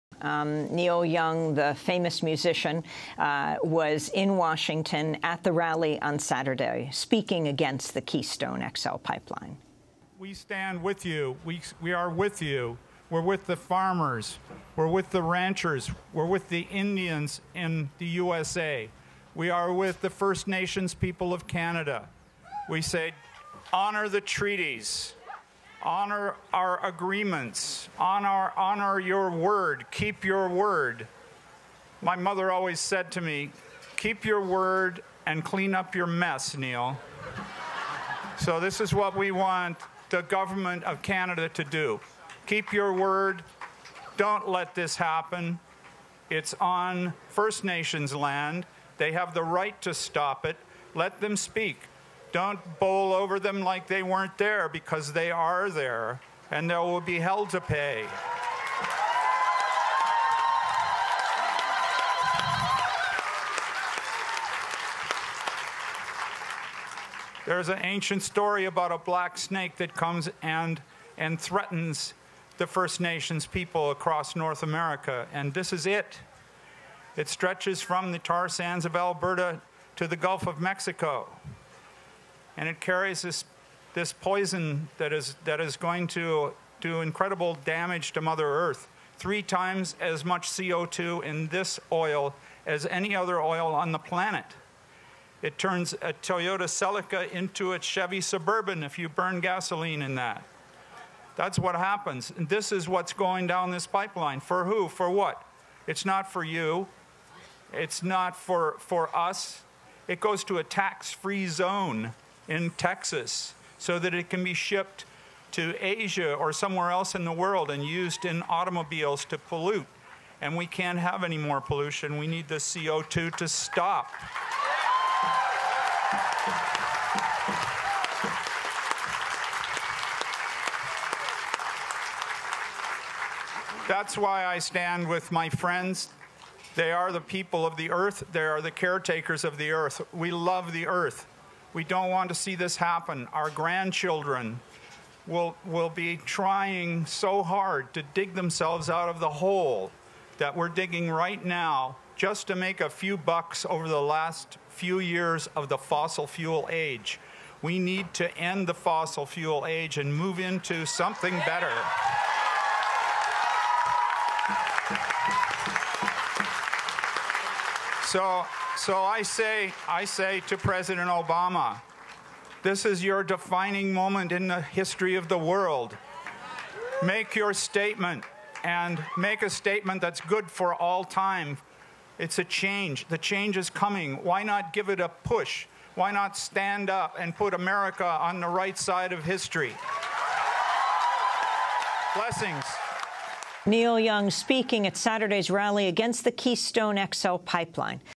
"Debemos detener la era del combustible fósil": Neil Young protesta contra el oleoducto Keystone XL
Lea/Vea el reporte completo en la web de Democracy Now! en Español

El legendario músico canadiense Neil Young se hizo presente en la manifestación contra el oleoducto Keystone XL, realizada en Washington DC el sábado 26 de abril de 2014.